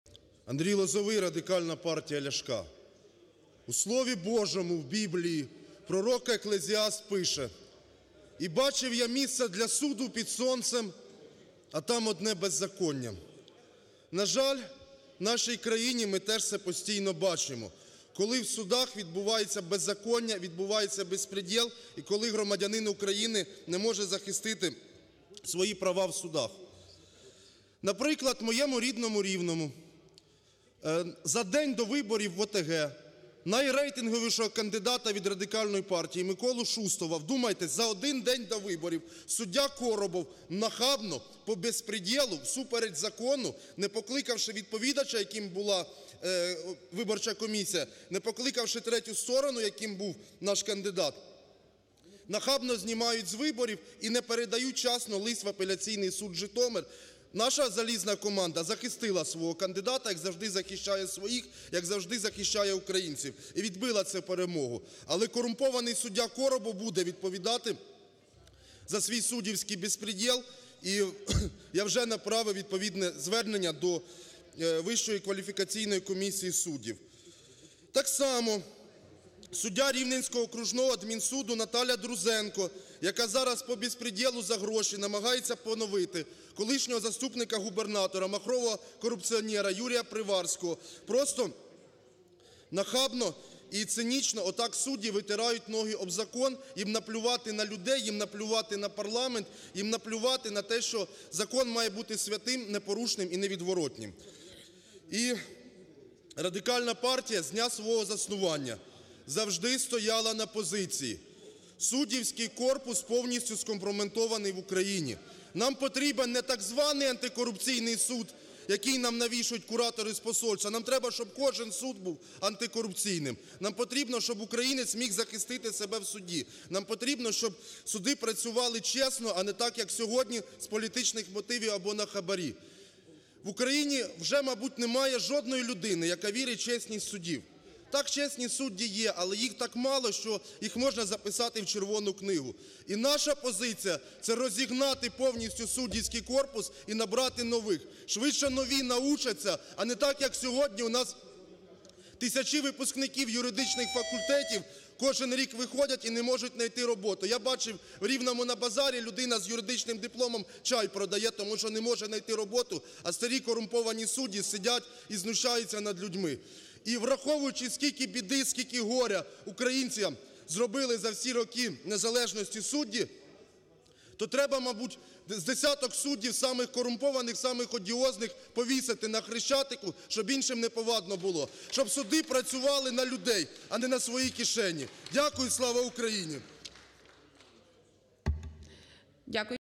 Лозовий розповів про суддівський бєспрєдєл на Рівненщині
Андрій Лозовий з трибуни парламенту розповів про спробу завадити Миколі Шустову стати головою Олександрійської ОТГ.  Політик переконаний у тому, що суддівський корпус повністю дискредитував себе. Народний депутат від Радикальної партії заявив, що потрібно звільнити усіх суддів і набрати нових.